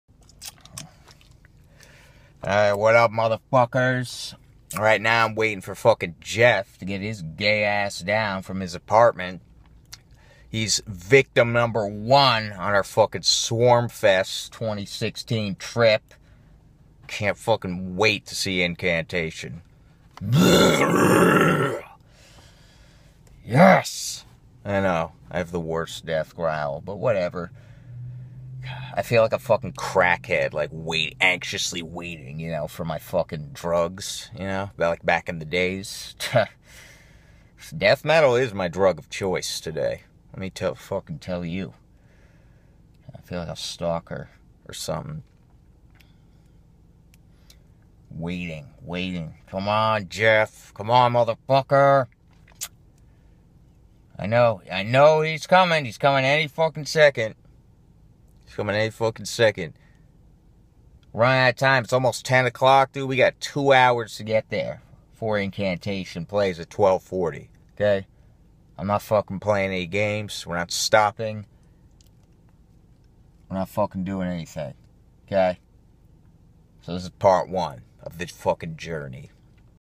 Trip to swarmfest part 1 11/19/16
I made a mistake about when incantation were playing because I'm a dumbass